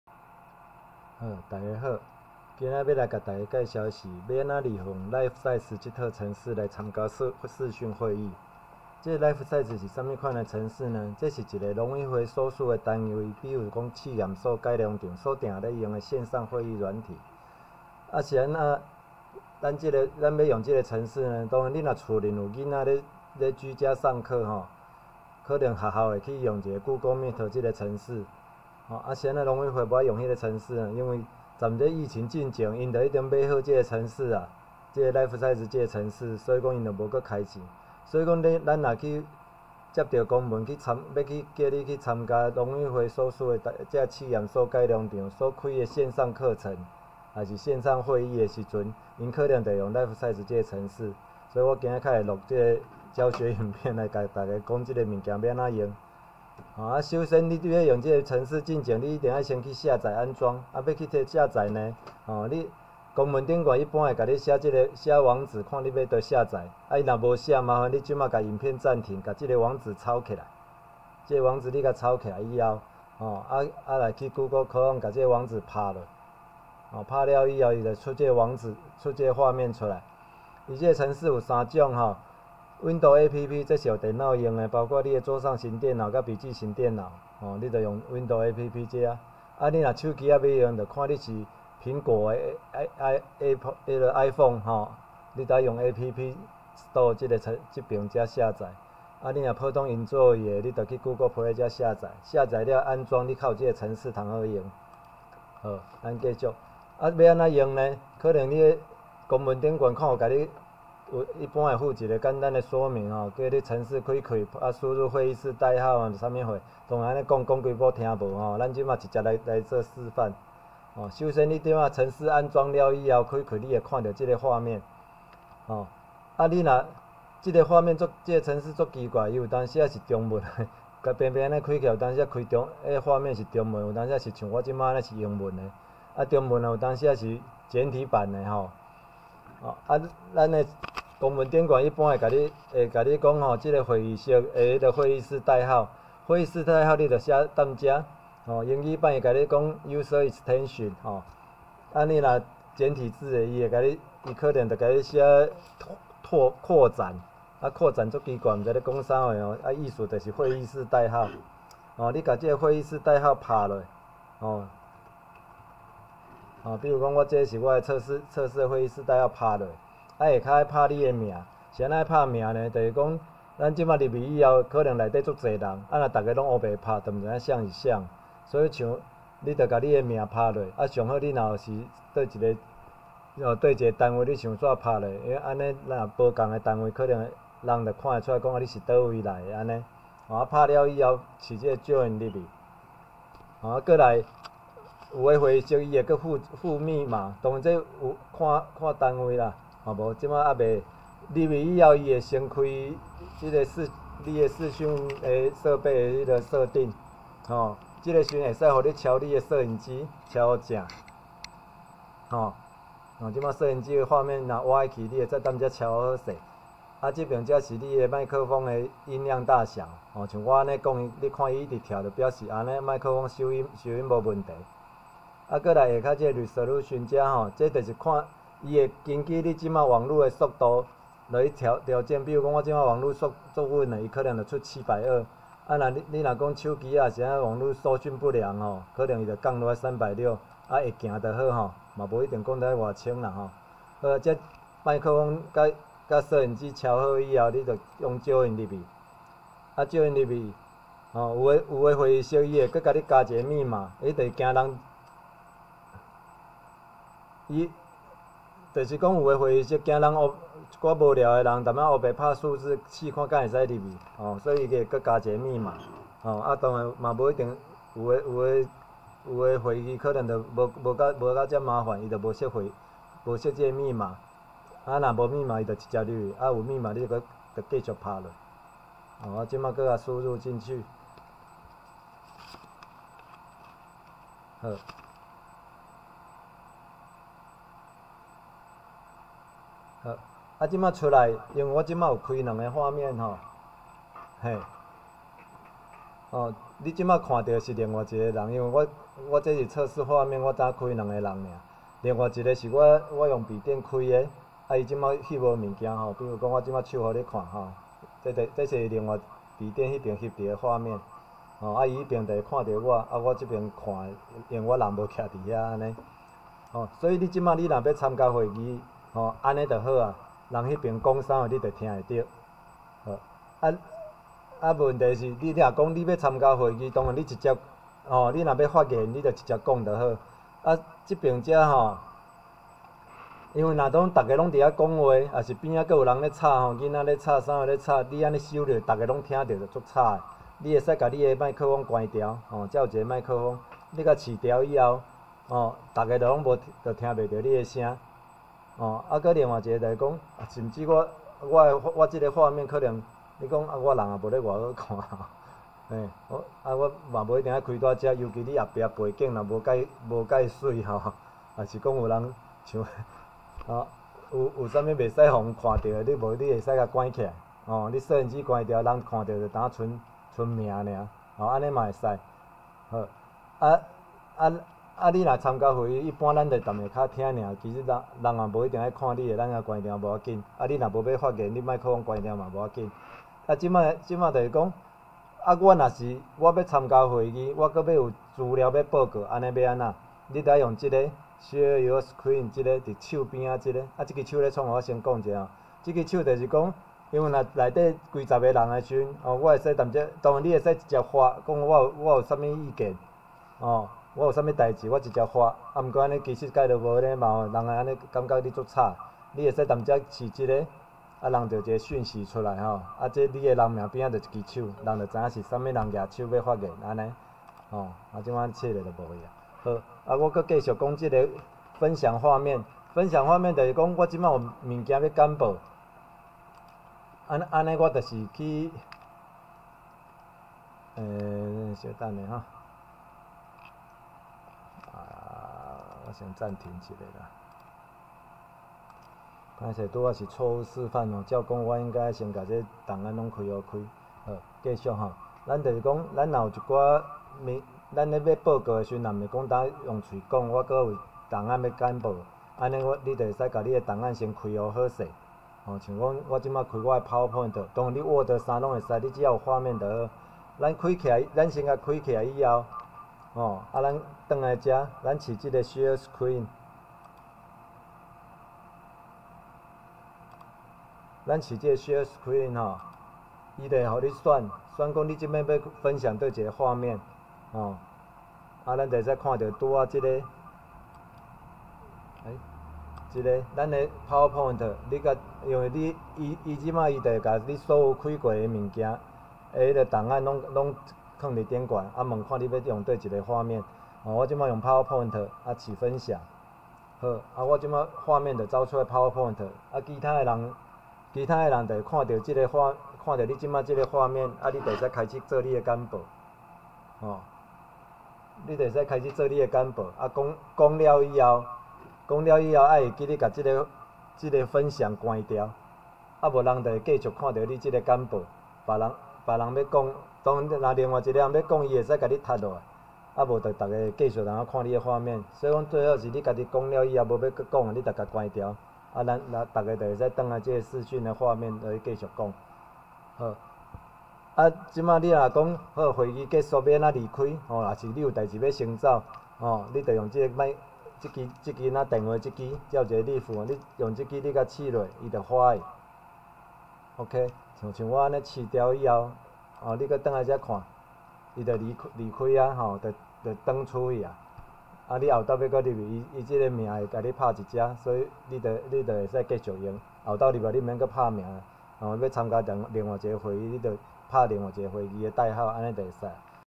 如何利用lifesize參加視訊會議
Lifesize是農委會所屬機關常用的視訊會議軟體，所以如果你需要參加農委會或者試驗所、改良場所舉辦的線上會議或者線上教學課程，可能就得用到這套軟體。所以我簡單錄了這個影片，為大家介紹這個軟體該怎麼用。